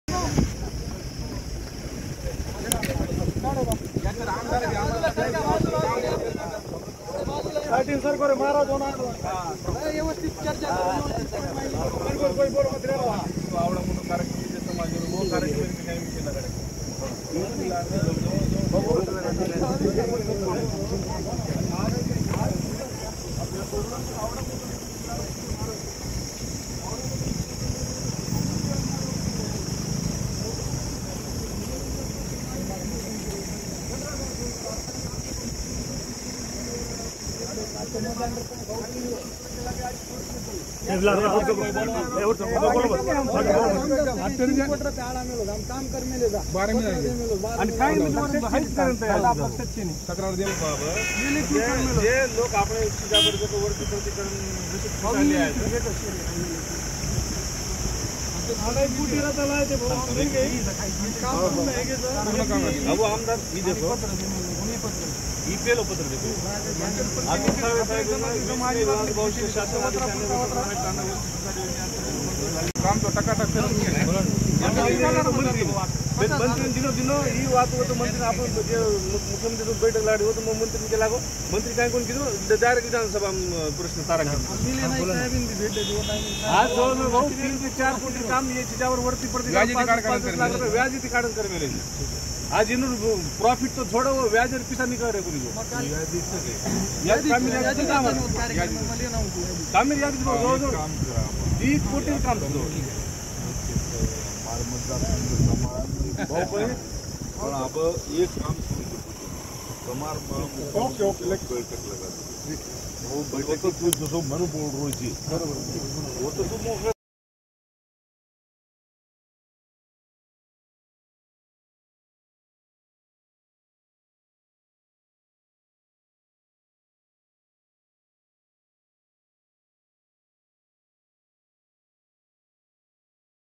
Sanjay Rathod | मंत्री संजय राठोडांना घातला घेराव | LetsUpp Marathi
मंत्री संजय राठोड बीड दौऱ्यावर असताना बंजारा ब्रिगेडच्या कार्यकर्त्यांनी त्यांना भर रस्त्यातच अडवलं. बंजारा समाजाच्या विविध मागण्यांचे निवेदन यावेळी देण्यात आले. यावेळी आश्वासन दिल्यानंतर पोलिसांच्या मध्यस्थीने त्यांचा ताफा पुढे निघाला.